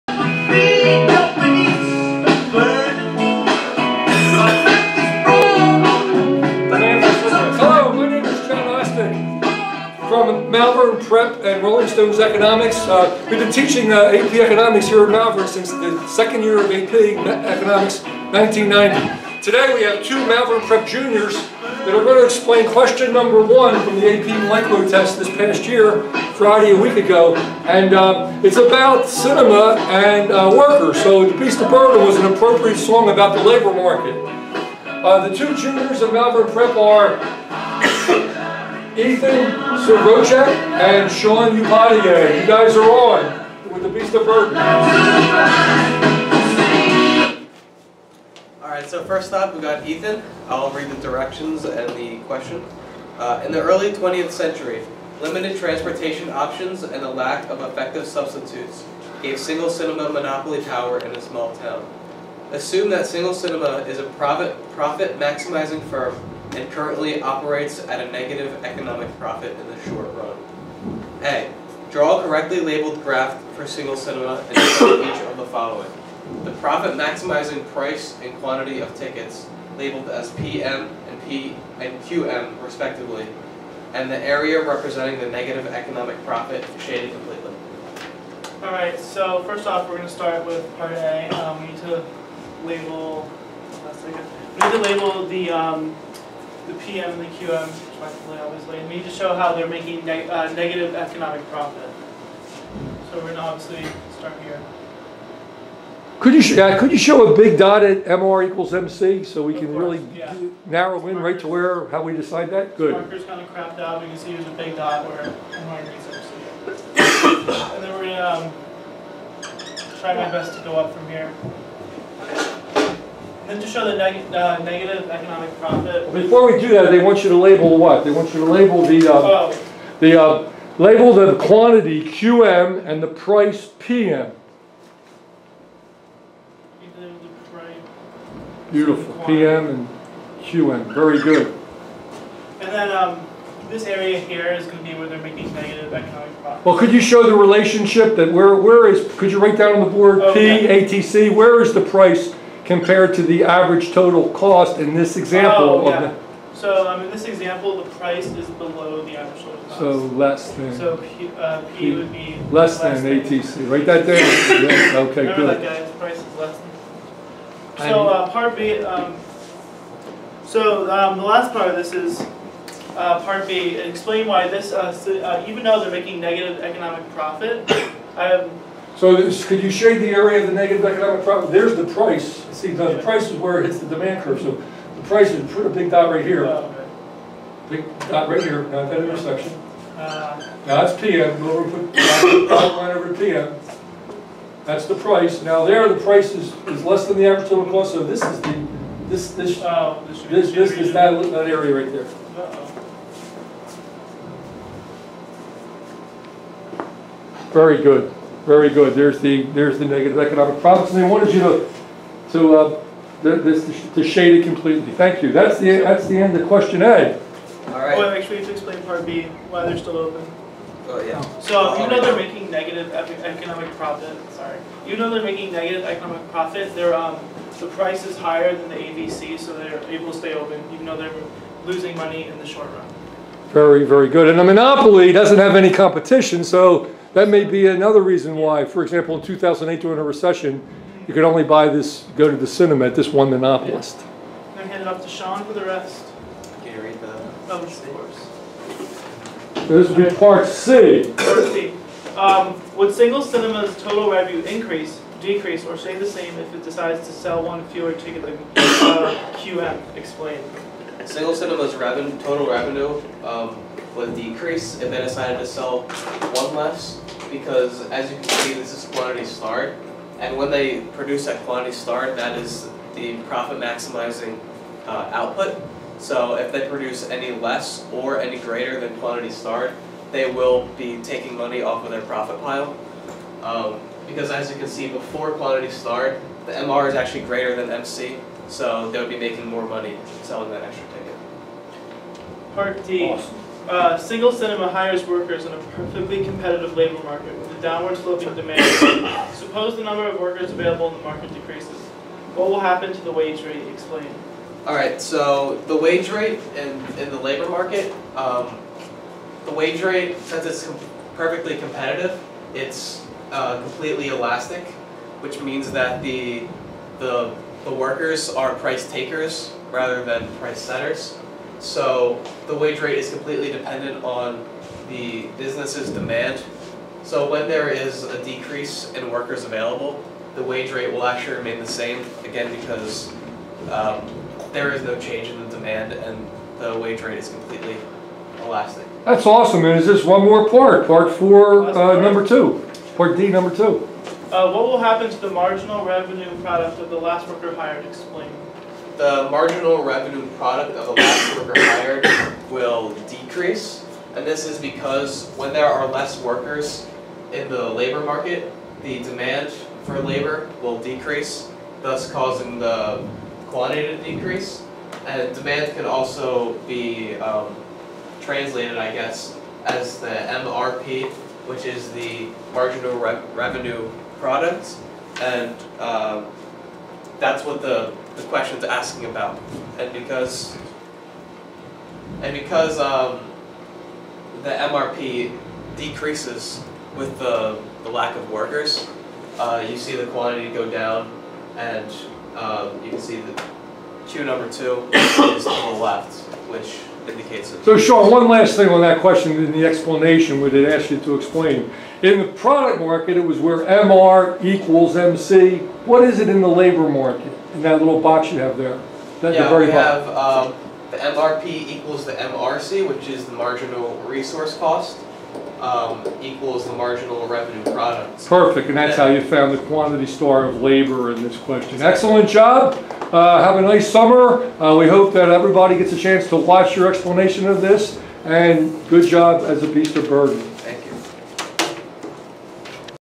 AP MICRO EXAM 2018 Scoring Rubric ----Free Response Question #1
2018 AP MICRO #1

Single Cinema, a monopoly with negative economic profit---

                          P less than  ATC at Q*

Presented by Malvern Prep Juniors:

Sean U
Ethan S